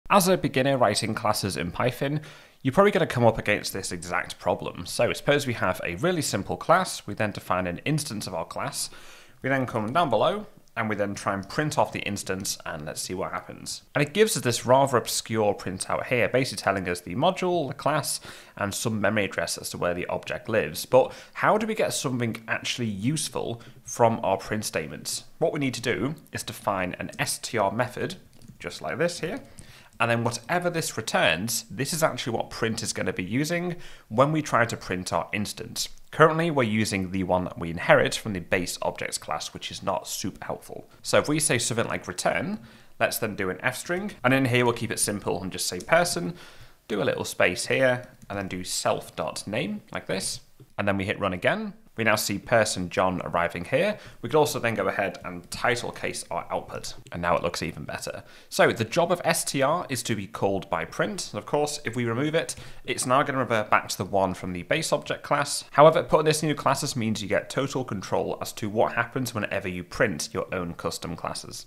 Python 3 | The Essential Magic Method!
One for beginners in OOP.  Here's a magic method you absolutely need!